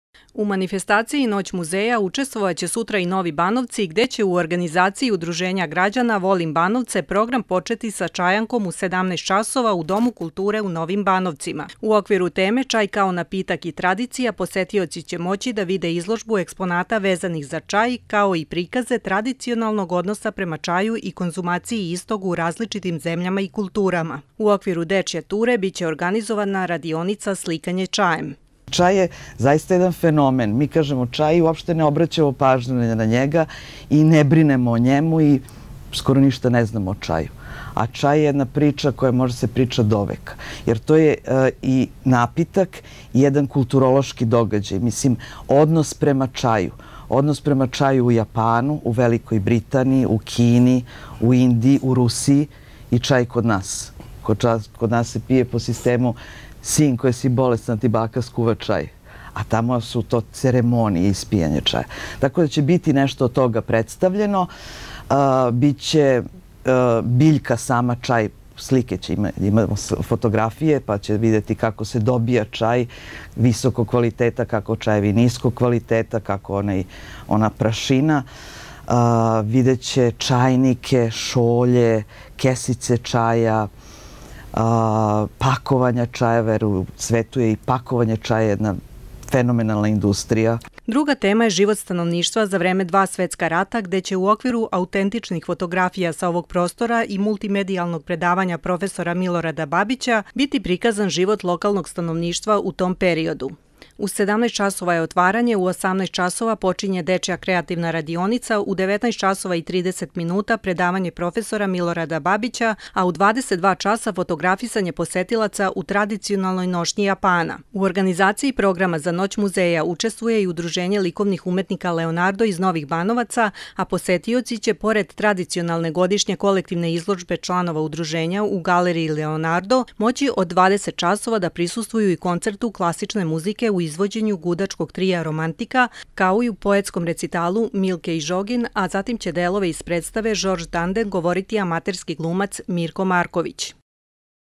Najava Noći muzeja
Novi Banovci su jedno od 60 mesta koja sutra  učestvuju  u tradicionalnoj  manifestaciji “Noć muzeja”, sa bogatim programom u organizaciji Udruženja građana “Volim Banovce” i  ULU “Leonardo” iz Novih Banovaca.